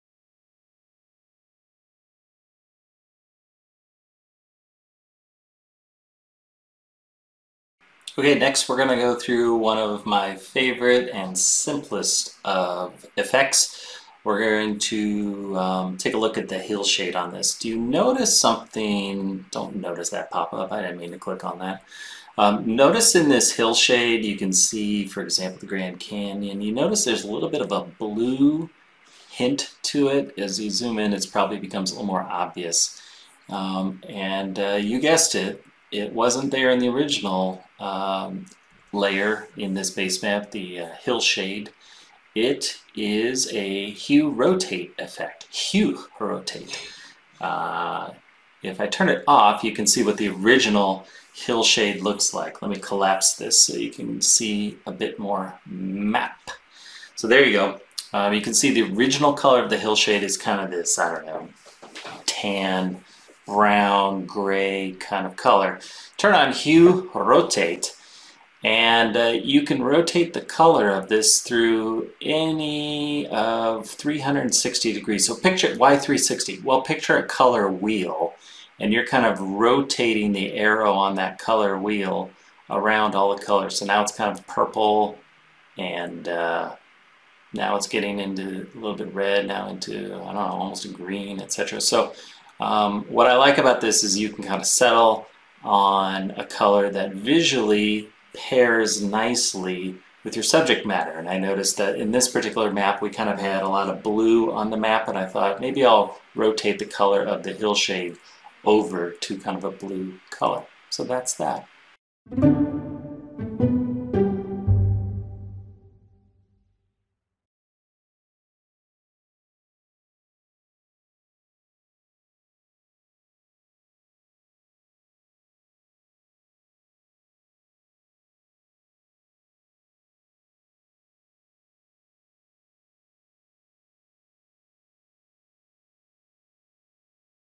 Map Viewer Effects: Hillshade Hue Rotate
With the September 2021 release of ArcGIS Online, we added a new capability to Map Viewer called Effects. Effects bring Photoshop-like capabilities to your layers in your web maps. ArcGIS Online makes it very easy to try out simple visual effects that make your maps stand out and get noticed. Sometimes your map needs just a little color adjustment to one layer or another. In this example, you’ll see how easy it is to rotate the hillshade layer’s default brownish-gray color into a soothing blue color that matches the map’s thematic topic.